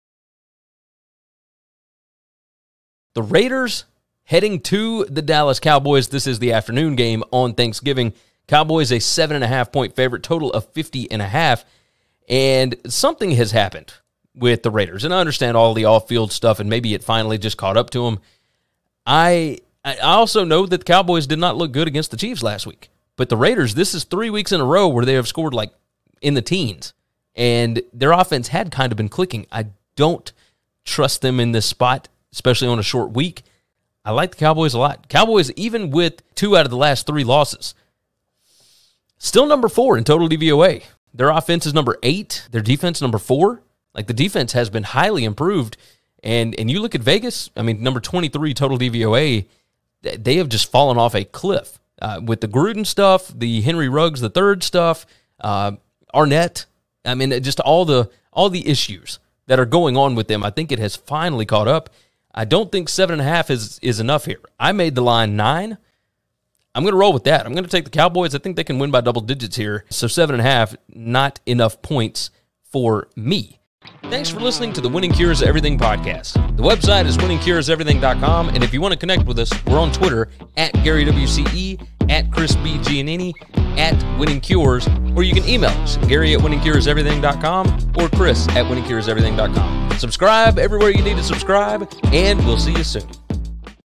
Las Vegas Raiders vs Dallas Cowboys 2021 NFL Week 12 Picks Against the Spread, Predictions
Gary & Chris give a quick Las Vegas Raiders vs Dallas Cowboys preview, predictions & their picks against the spread (ATS) for the NFL Week 12 matchup this weekend!